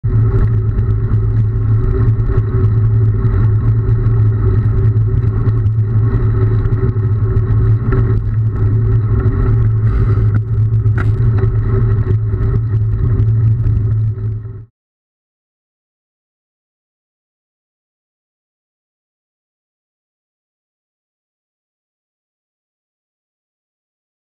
car on wrong side of the road
Cycling on Cumbrae enjoying a sunny winters day when a car crosses onto the wrong side of the road and heads straight for me.